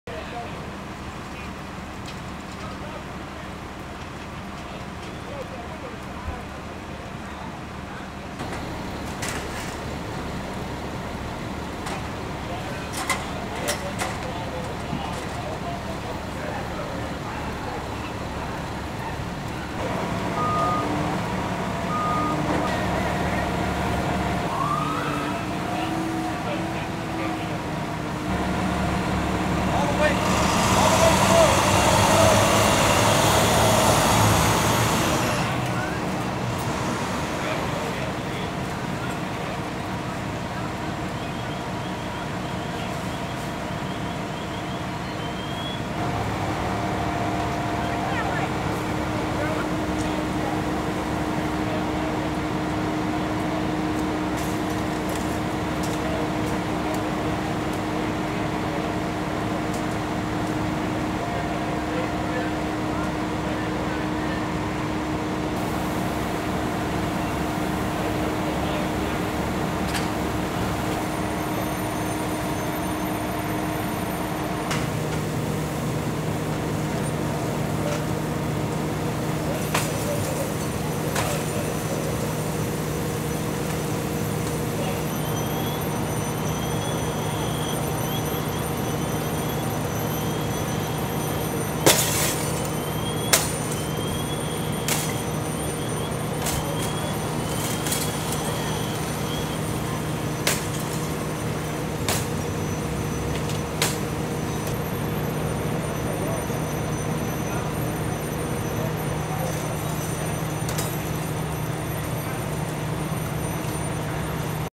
Fatal Cleveland fire
A man has died in an apartment fire at the intersection of East 79th and Linwood in Cleveland. A female was taken to MetroHealth Medical Center with burn injuries.